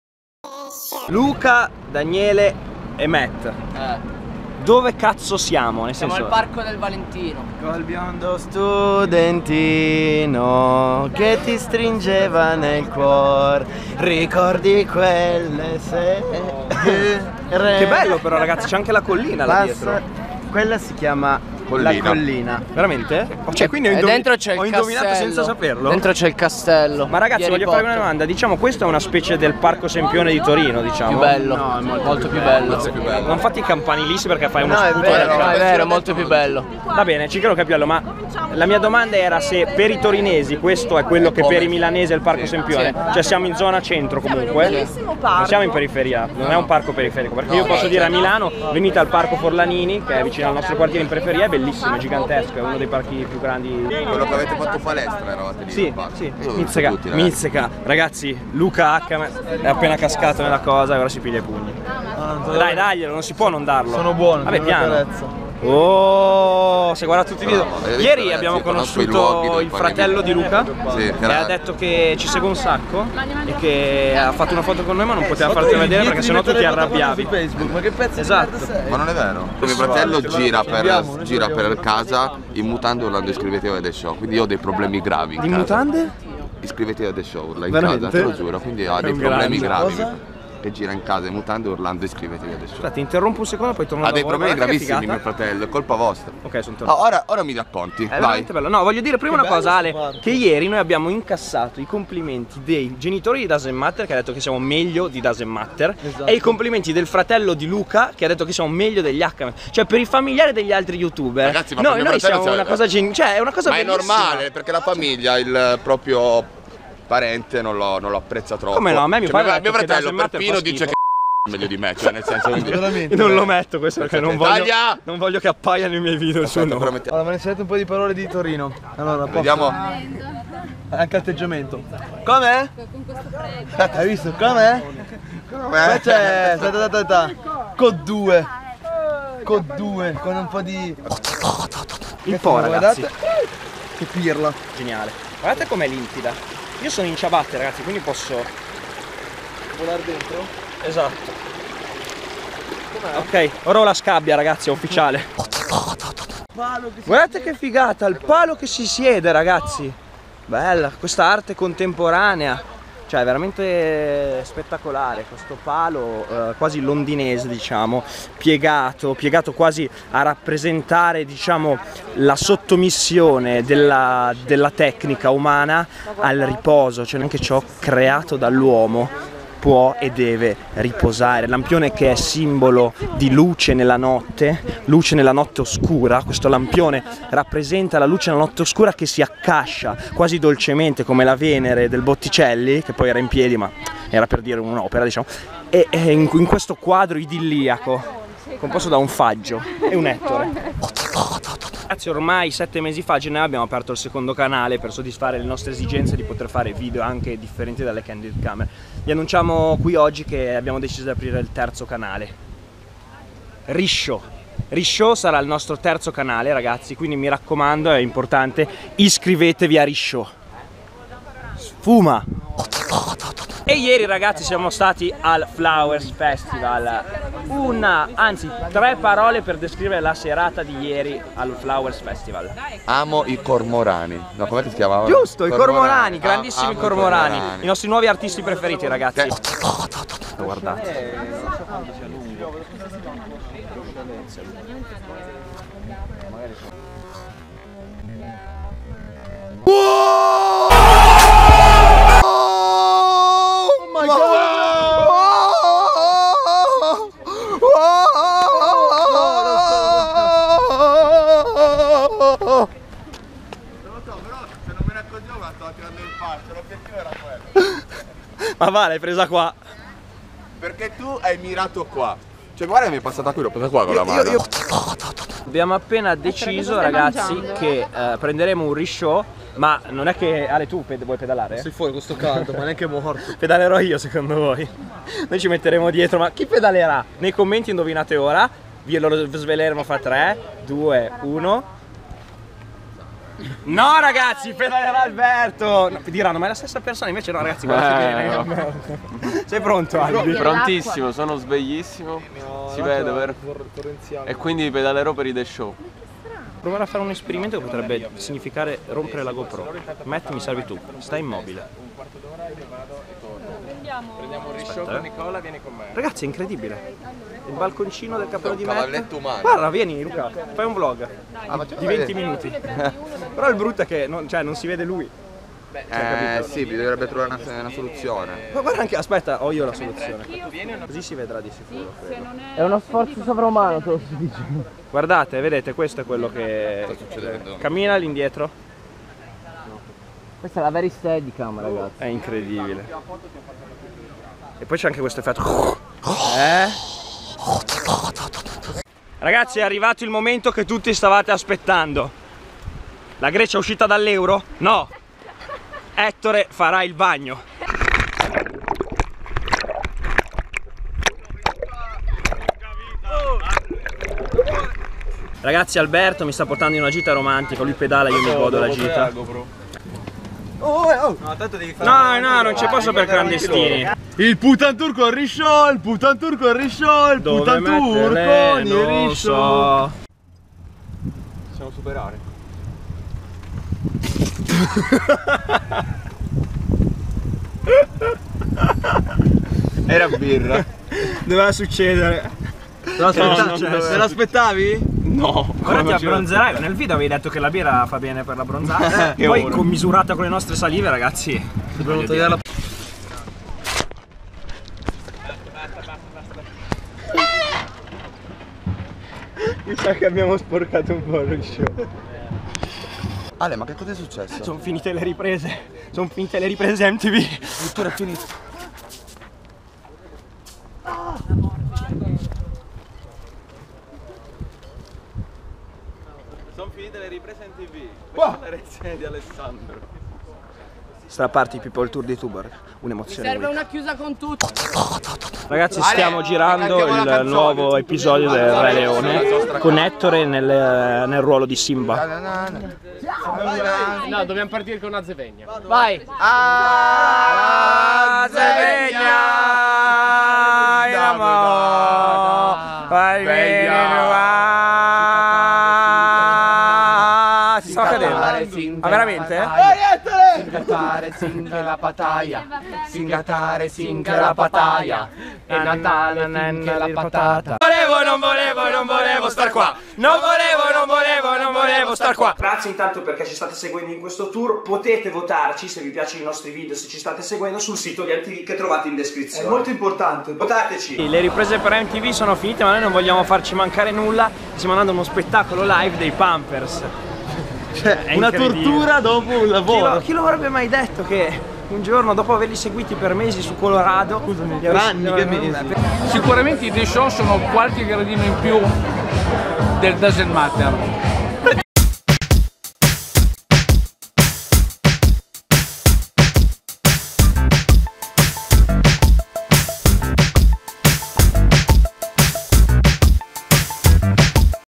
Il Terzo Canale dei theShow - THESHOW IN TOUR #17 [Torino] - deSciò
A 20'000 LIKE PROSSIMO EPISODIO!
Il nostro tour sta volgendo al termine! Si concludono le riprese per MTV e noi approfittiamo dell'occasione per goderci una gita romantica in risciò con Alberto...